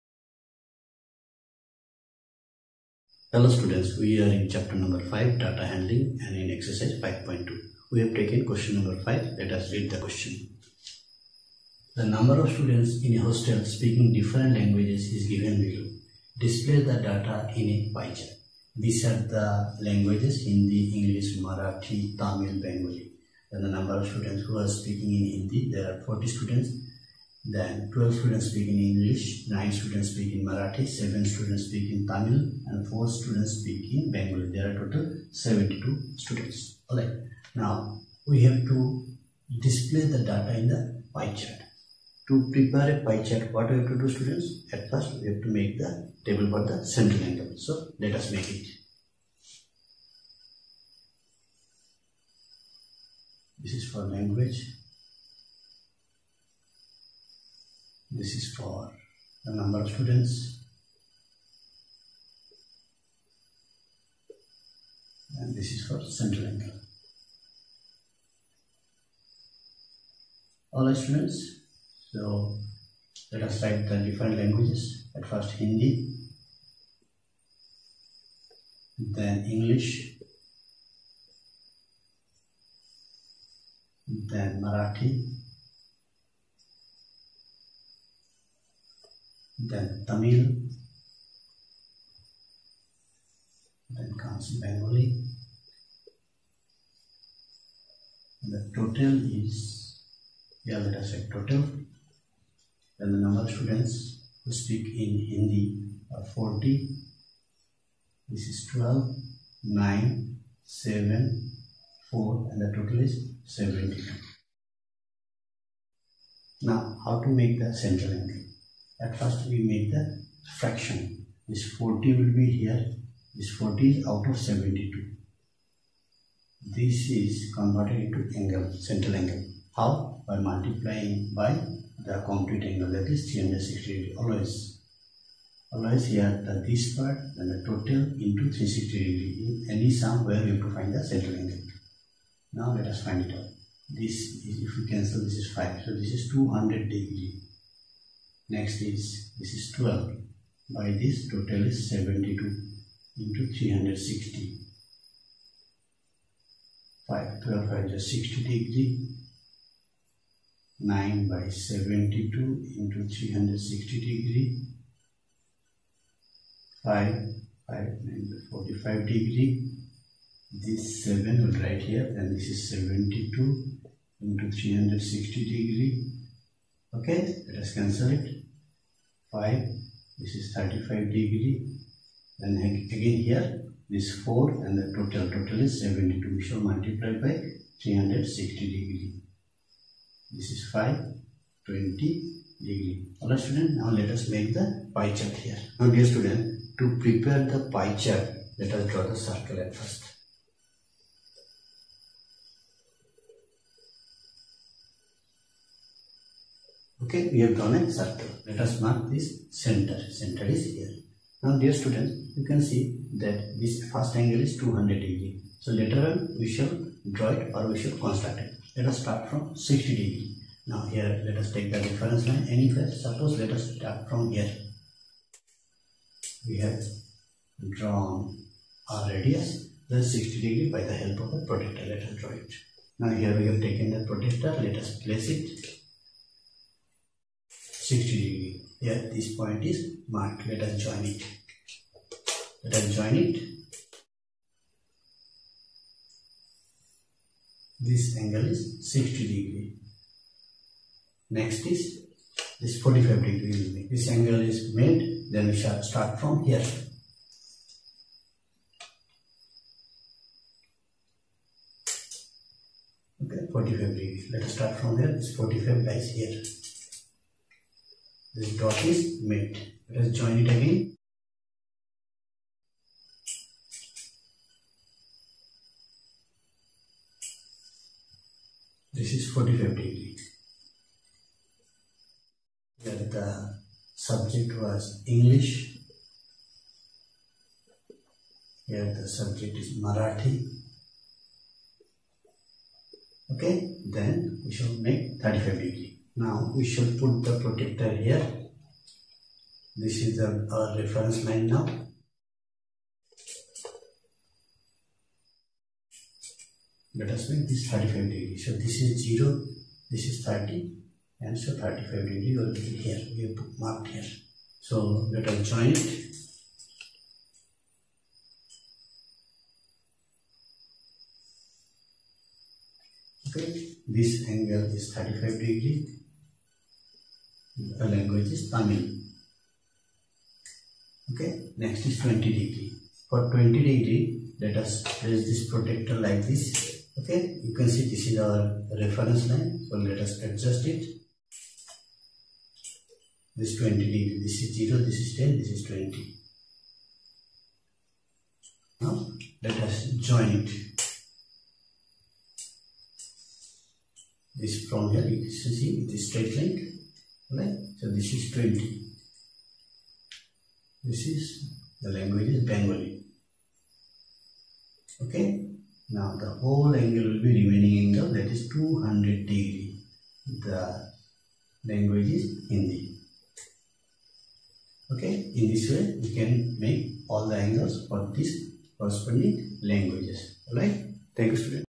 Q 5 of Exercise 5.2 from the Chapter Data Handling of class 8 Mathematics
Question number 5 of the chapter Data Handling of class 8 Mathematics under NCERT is discussed here